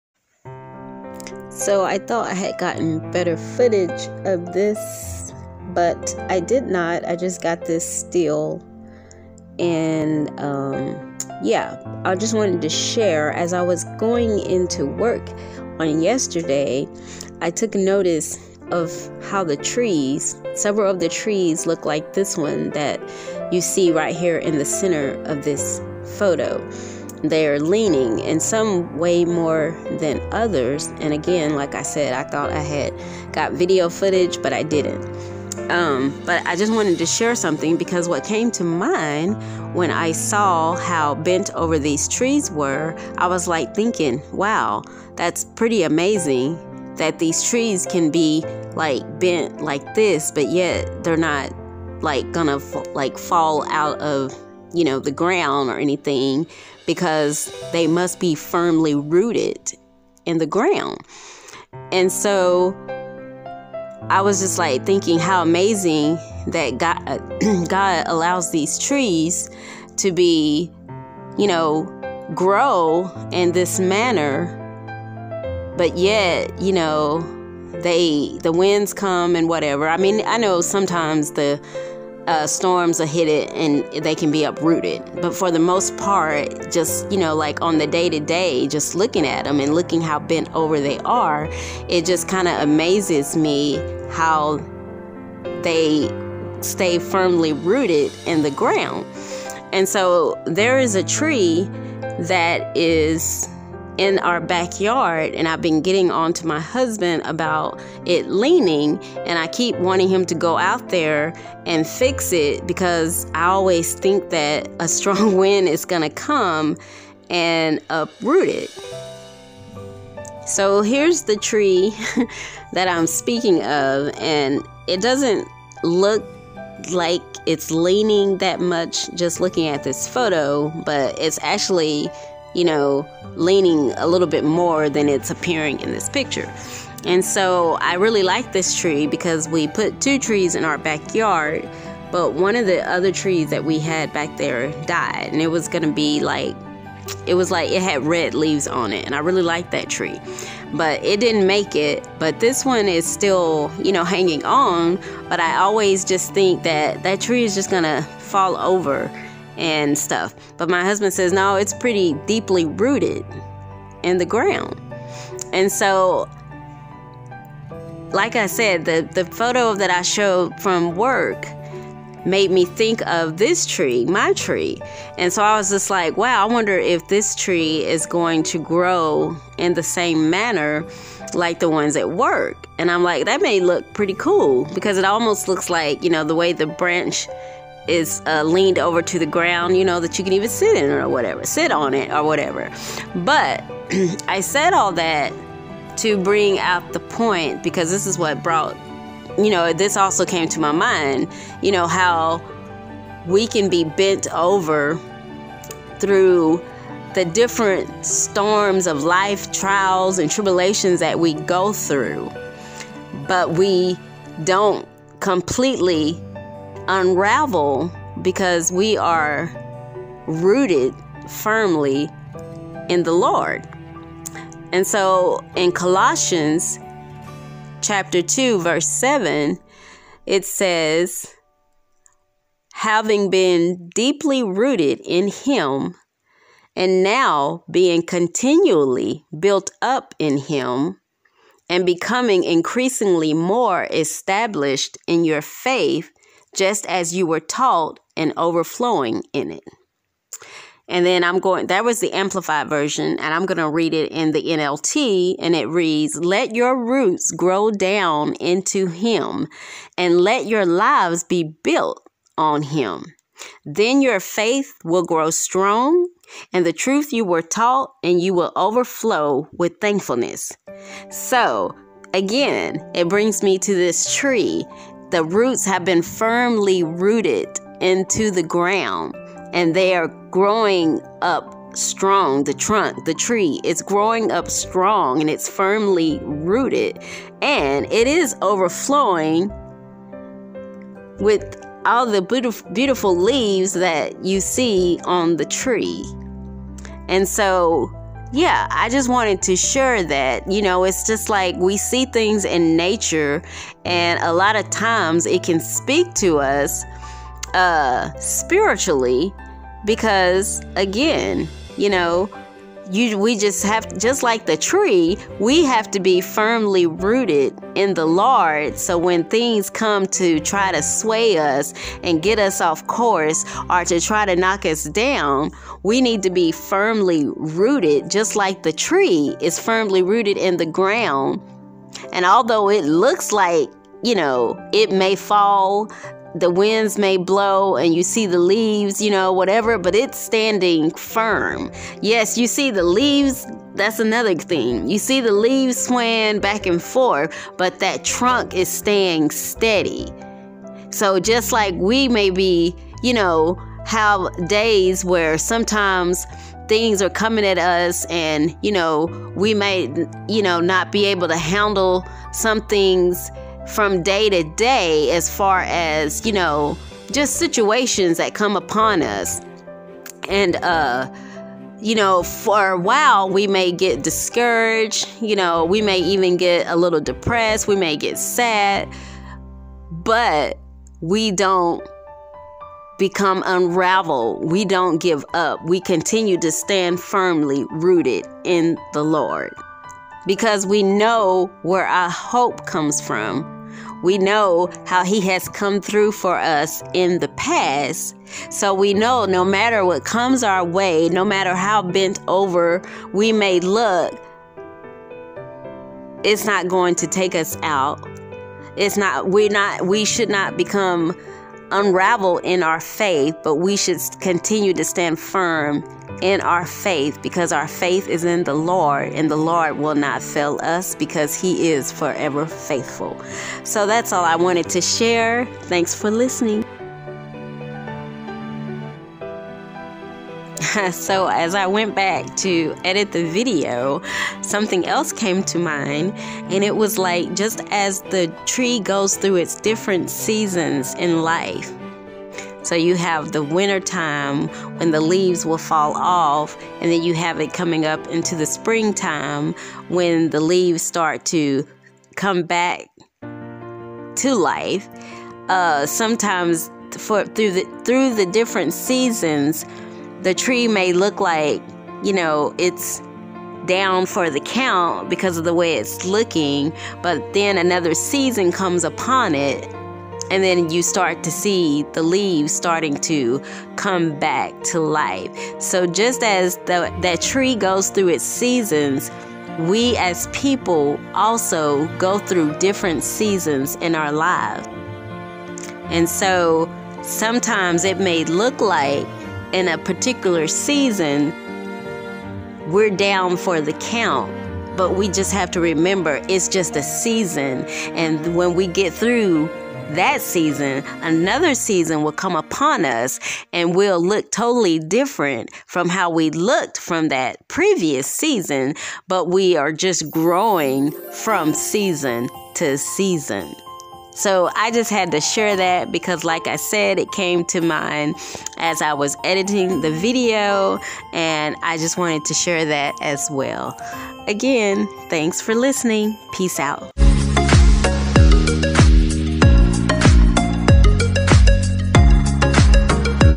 Firmly Rooted
just sharing a thought that came to mind when I walked in to work the other day and noticed these bent over trees.

Love❤ Peace😇 Blessings🙏🏾